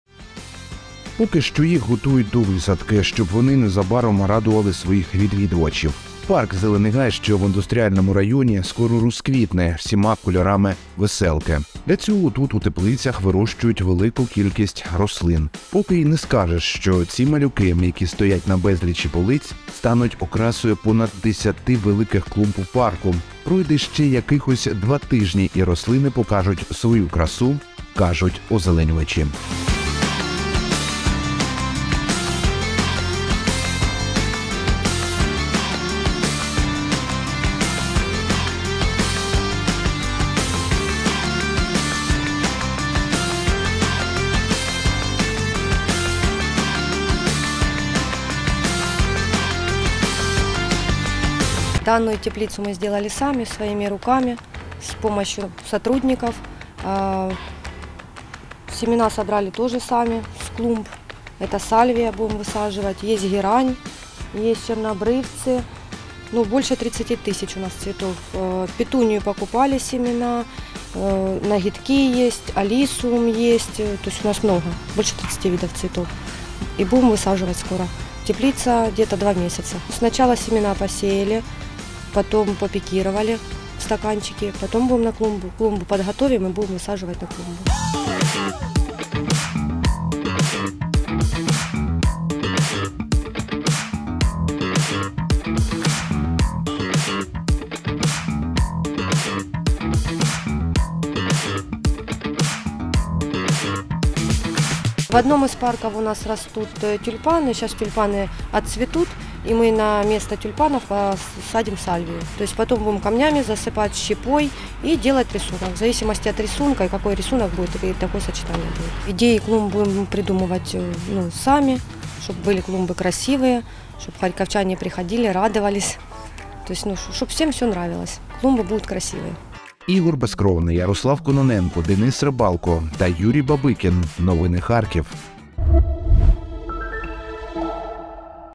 Десятки тисяч рослин вирощують для квітників у парку «Зелений гай»
Поки що їх тільки готують до висадки. Щоб вони незабаром радували своїх відвідувачів. Парк "Зелений гай", що в Індустріальному районі скоро розквітне всіма кольорами веселки. Для цього тут у теплицях вирощують велику кількість рослин. Поки й не скажеш, що ці малюки, які стоять на безлічі полиць, стануть окрасою понад 10 великих клумб у парку. Пройде ще якихось два тижні і рослини покажуть свою красу, кажуть озеленювачі.